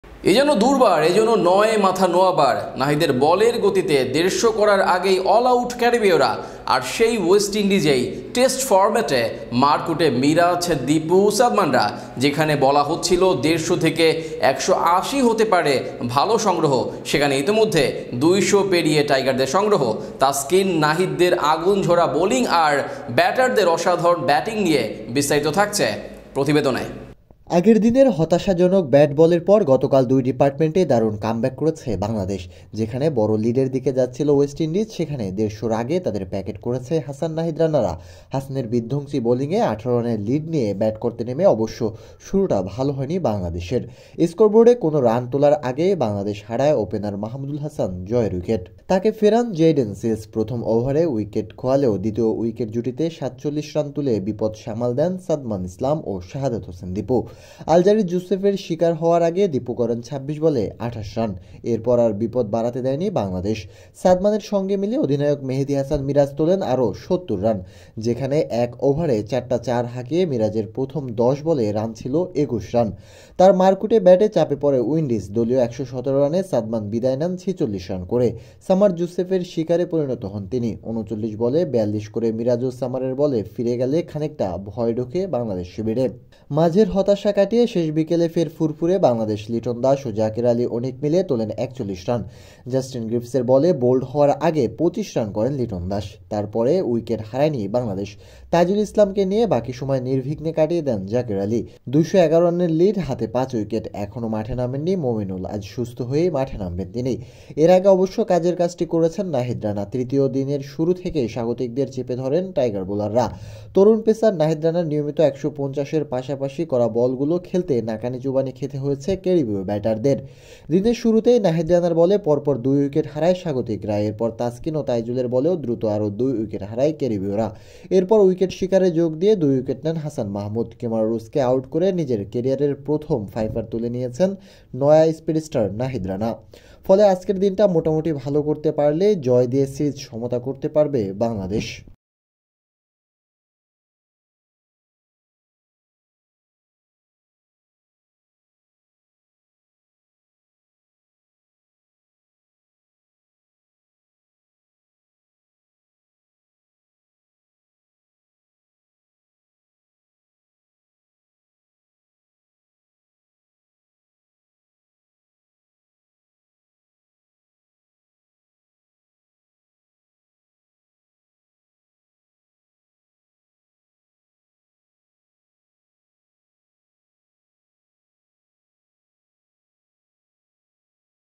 ৪,৪,৪,৪ টেস্ট ম্যাচেই এক ওভারে মিরাজের চার চার,নাহিদের ৫ উইকেট!
৪,৪,৪,৪ টেস্ট ম্যাচেই এক ওভারে মিরাজের চার চার,নাহিদের ৫ উইকেট!

৪,৪,৪,৪ টেস্ট ম্যাচেই এক ওভারে মিরাজের চার চার,নাহিদের ৫ উইকেট!